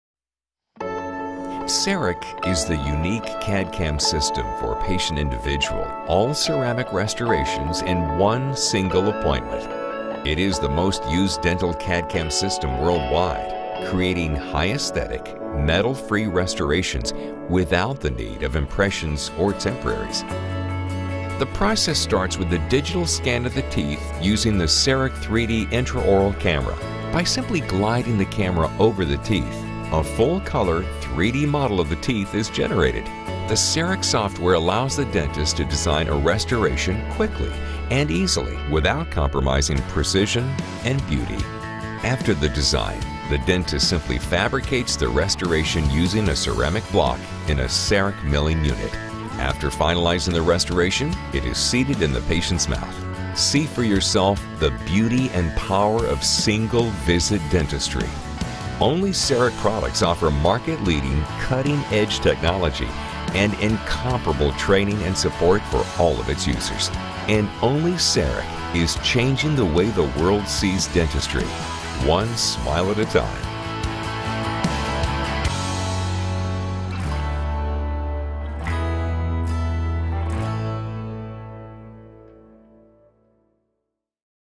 CEREC Cadcam System for All Ceramic Restorations in a Single Visit
Learn more about the CEREC Cadcam System and how it can help you get all ceramic restorations in a single visit.